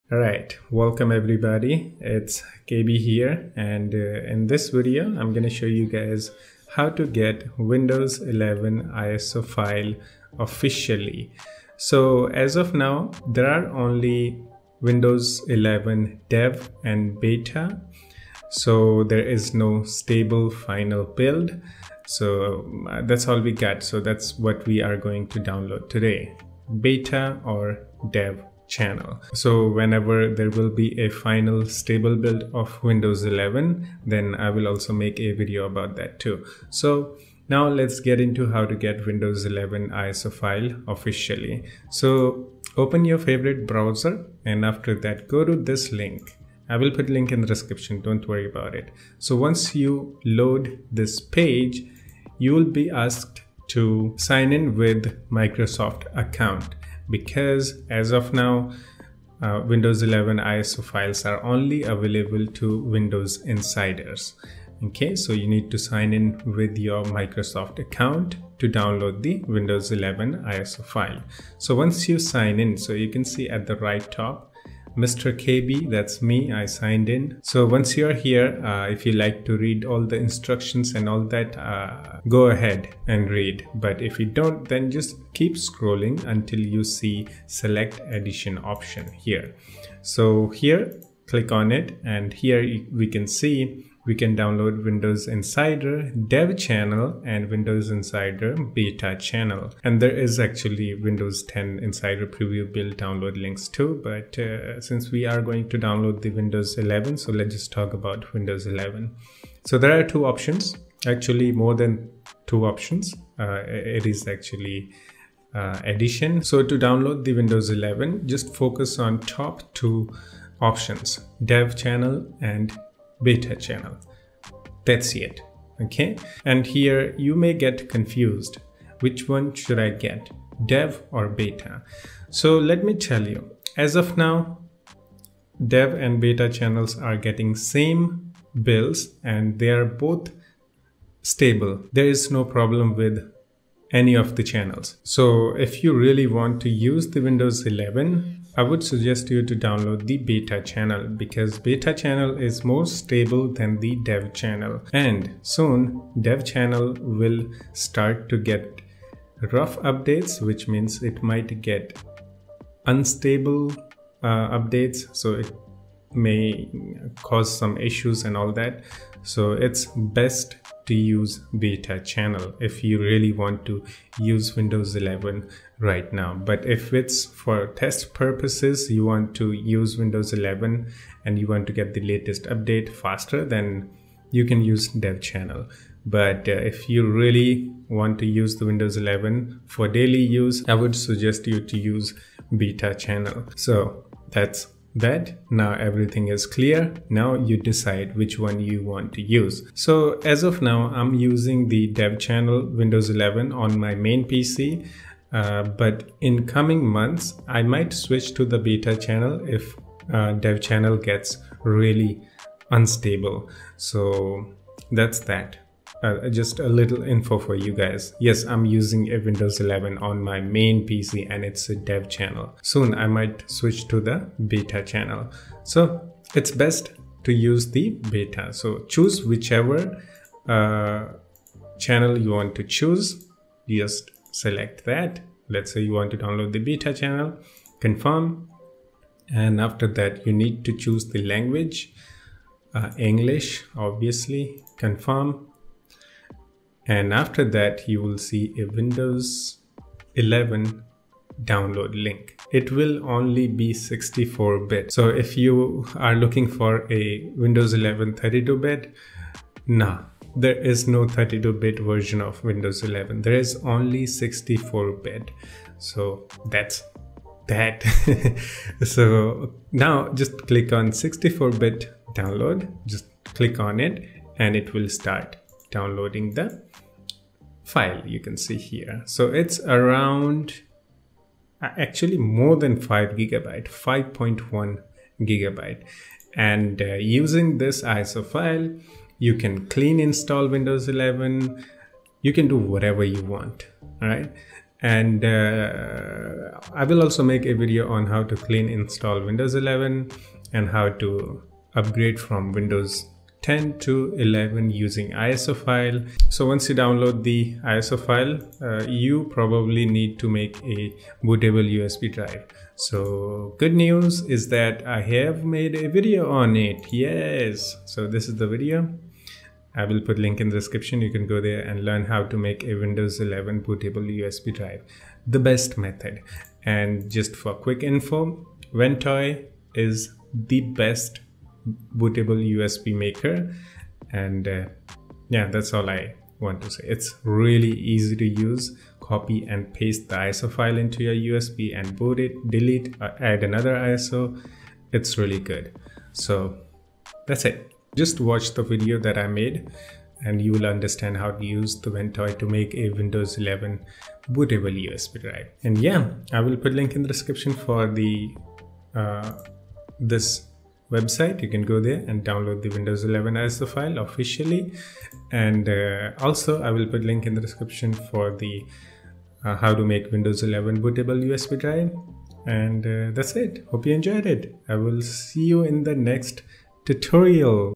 How To Get Windows 11 ISO Officially (Beta and Dev)
Windows 11 preview builds Dev and Beta channels ISO files are now officially available for downloads...